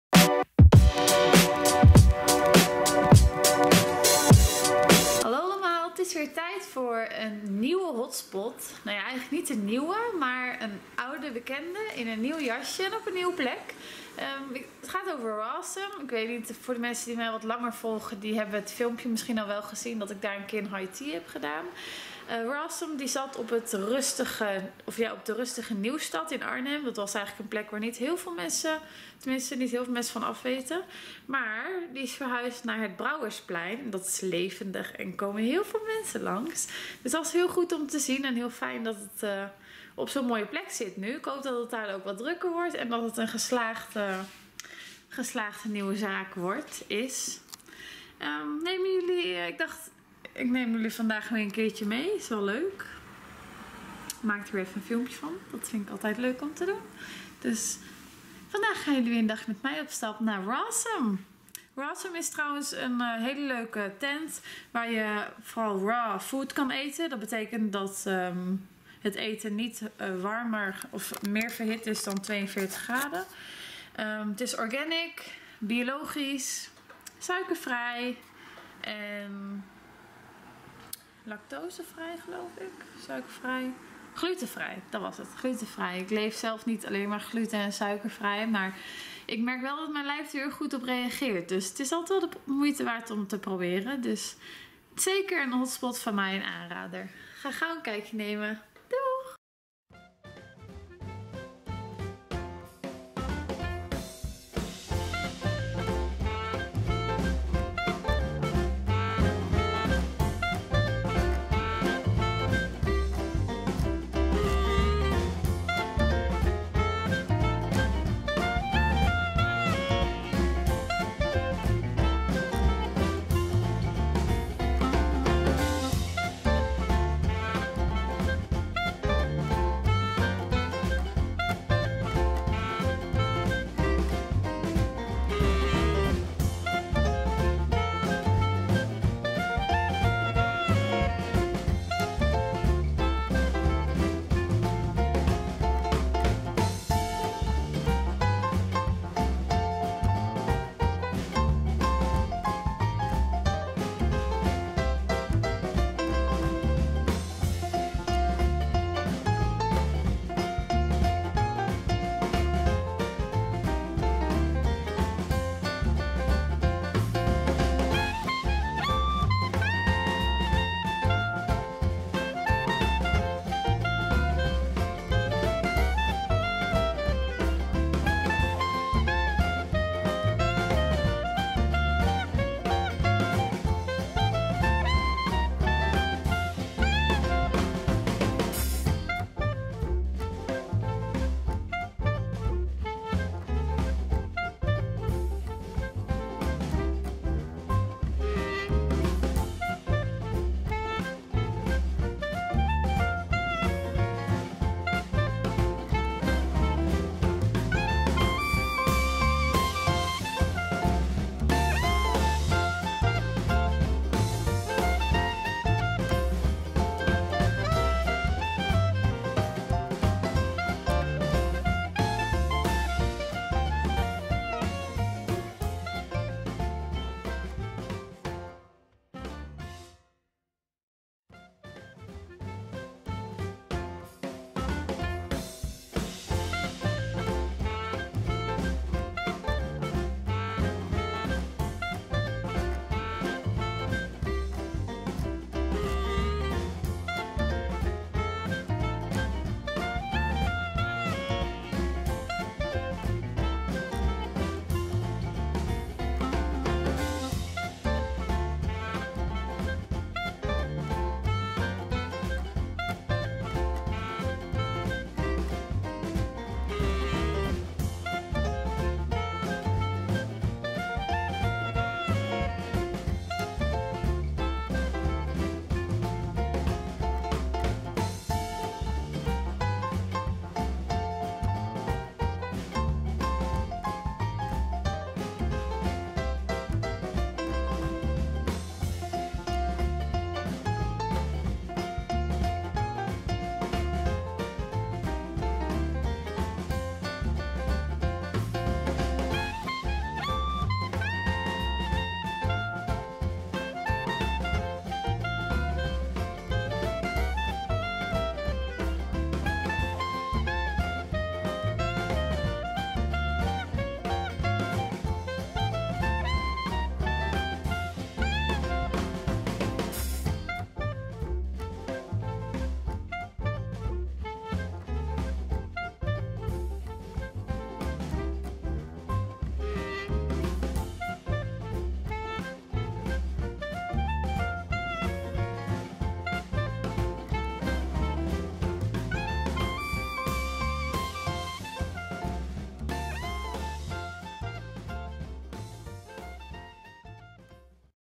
UPDATE! Rawsome Arnhem.
Zoals sommige van jullie misschien wel weten heb ik een tijdje geleden een video opgenomen bij Rawsome in Arnhem. Rawsome is verhuisd, ze zijn nu gevestigd op het levendige Brouwersplein en eerlijk is eerlijk, ik ga nu zelf ook veel vaker langs omdat het op de looproute ligt en ik er veel sneller langs kom.

Rawsome is Vegan, Raw, Organic, Glutenvrij etc.

Bedankt voor het kijken, neem zeker is een kijkje en doe Sharon de groetjes ;)